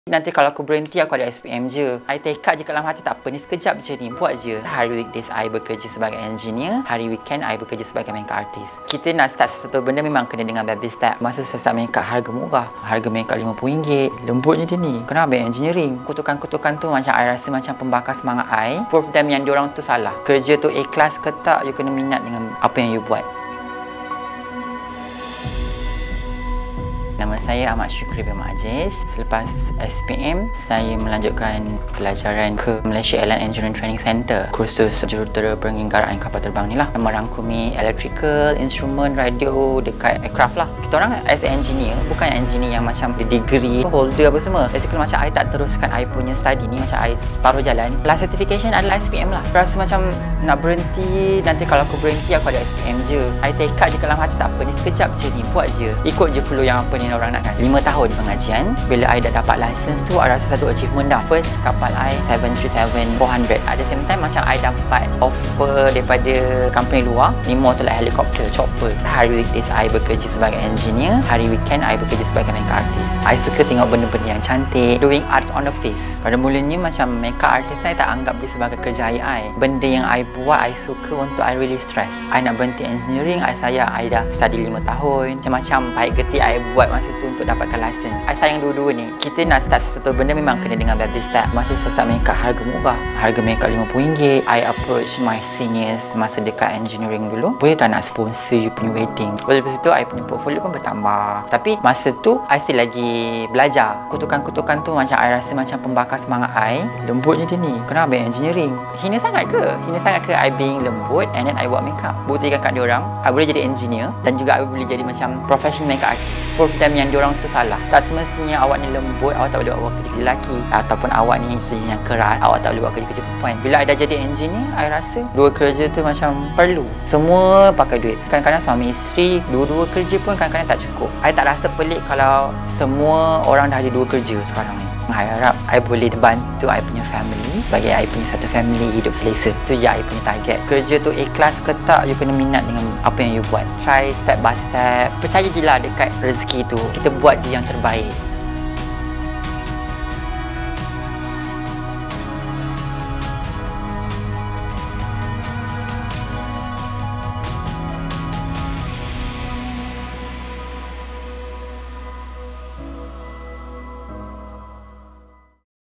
mausembang | Engineer pesawat 'part time' jadi jurusolek - Cuki Ajis
Bidang engineering dan solekan adalah kerjaya yang dimiliki oleh Syukry Md Ajis. Kini, dia membuktikan tiada apa yang mustahil jika ada usaha dan minat.